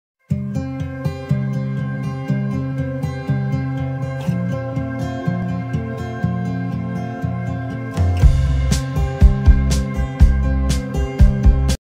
NOVOTECH KIT 358530 358531
Низковольтная (48В) трековая система освещения KIT. Это новейшая замена магнитной трековой системы. В ней используется конструкция защелкивающегося соединения, чтобы зафиксировать светильник на треке.